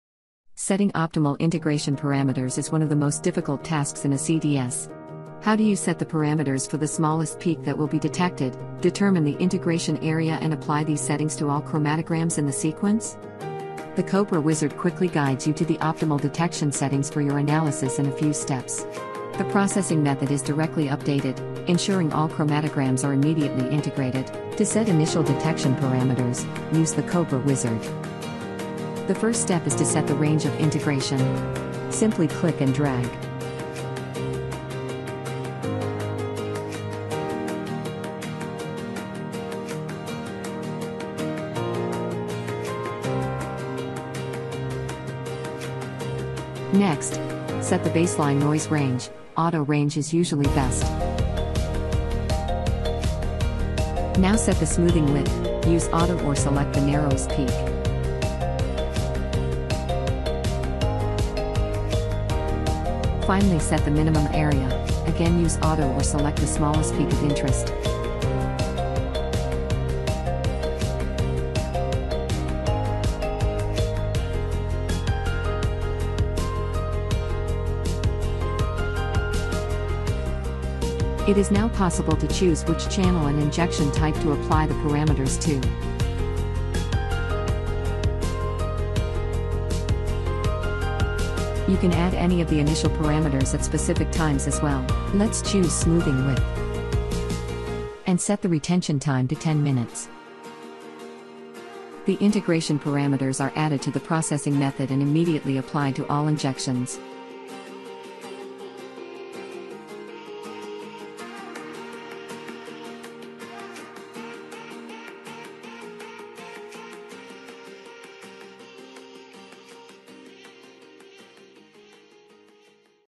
Thermo Scientific Chromeleon Cobra Wizard
The Cobra wizard quickly guides you to the optimal detection settings for your analysis in a few steps. The processing method is directly updated, ensuring all chromatograms are immediately integrated.